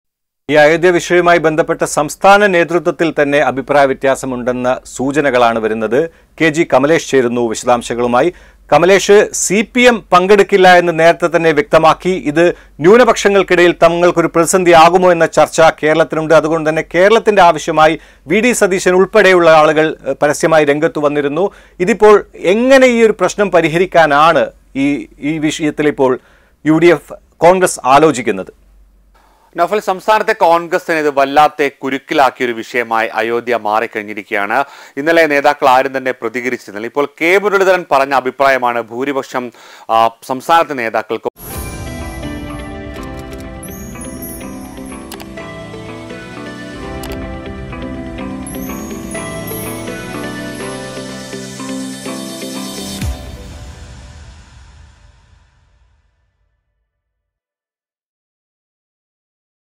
അയോധ്യ വിഷയം, സംസ്ഥാന കോൺഗ്രസിൽ ഭിന്നത | Ayodhya Ram Temple
അയോധ്യയിലെ പ്രതിഷ്ഠാ ചടങ്ങളിൽ കോൺഗ്രസ് പങ്കെടുക്കരുതെന്ന് കെ മുരളീധരൻ, സിപിഎം കേരളത്തിൽ ആയുധമാക്കുമെന്ന് കോൺഗ്രസ്,അയോധ്യ വിഷയത്തിൽ കേരളഘടകത്തിൽ ഭിന്നത
Ayodhya Ram Temple | Congress  | UDF